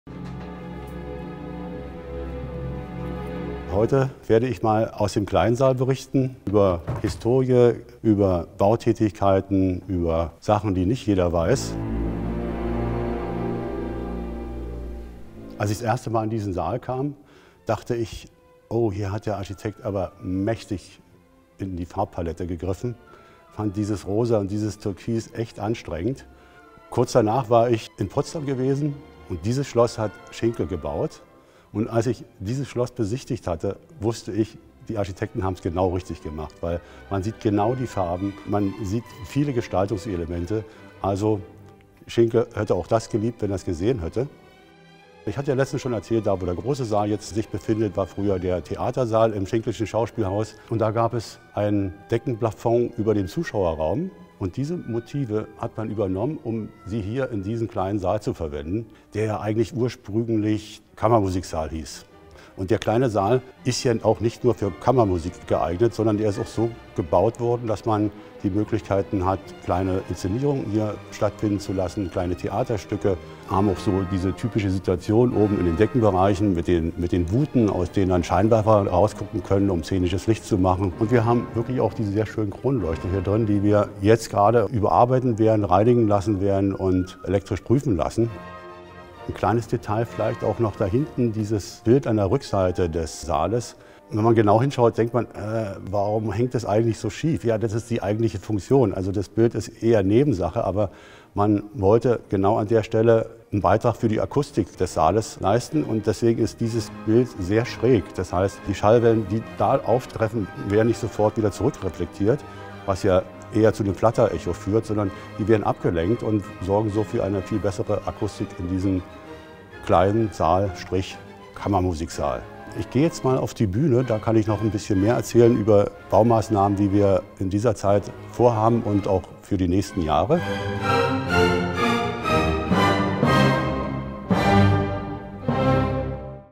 konzertZUhaus Einblicke | Der Kleine Saal
Klein aber oho! Was unser Kleiner Saal außer Kammermusik noch so alles kann, erzählt unser Technischer Direktor Peter Laduch in der neuen Folge „Einblicke“.

🤔 Was ist eigentlich gerade im Konzerthaus am Gendarmenmarkt los? Unser Technischer Direktor Peter Laduch führt durch das aktuell leere Gebäude und gibt Backstage-Einblicke. 🏛